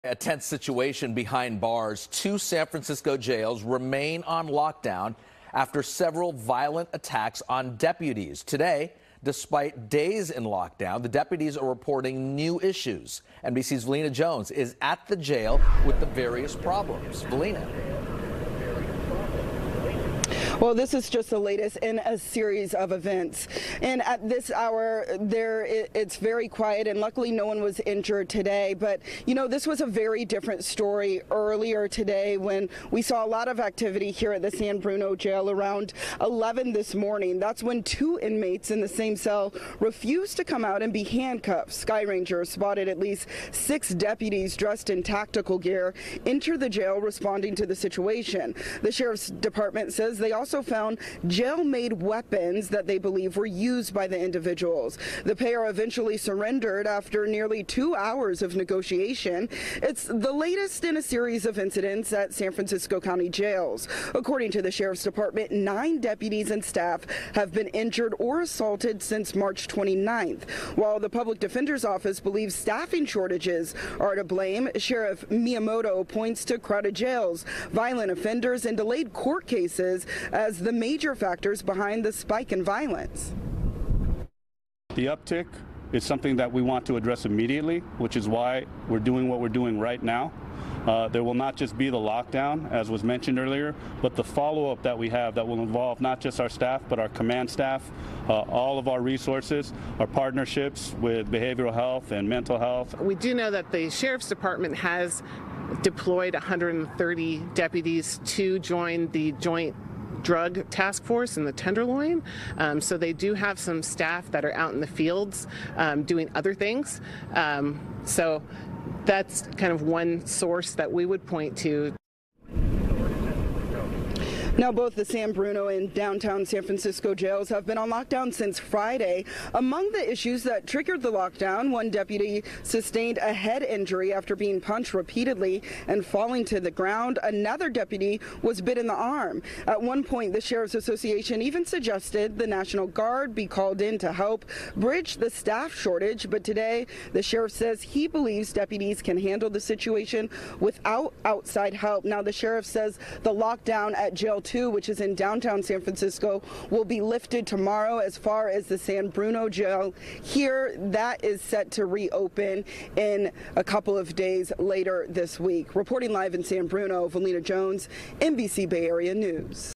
Violent attacks on deputies continue despite lockdown at San Francisco jails
Two San Francisco jails remain on lockdown Tuesday after several violent attacks on deputies. However, despite days in lockdown, deputies are reporting new issues. Velena Jones reports.